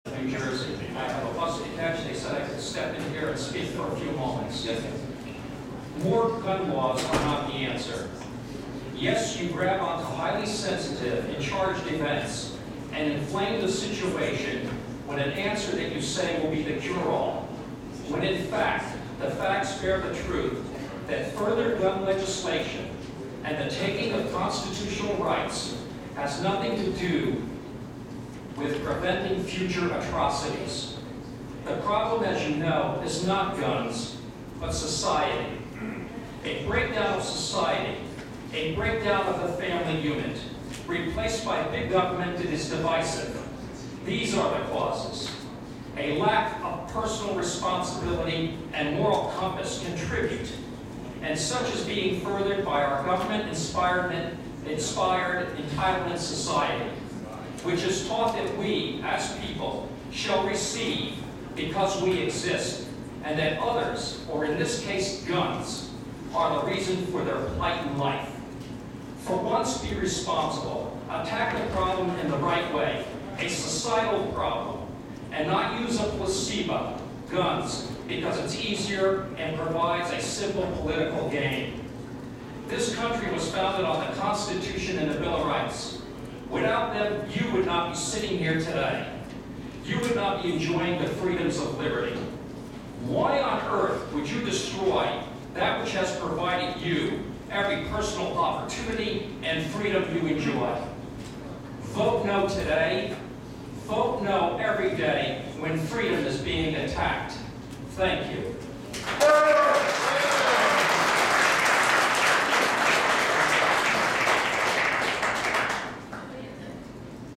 NJ Citizen Lectures Assmebly Gun Control Committee - Feb. 13, 2013
This articulate citizen explains how gun laws will not reduce crime.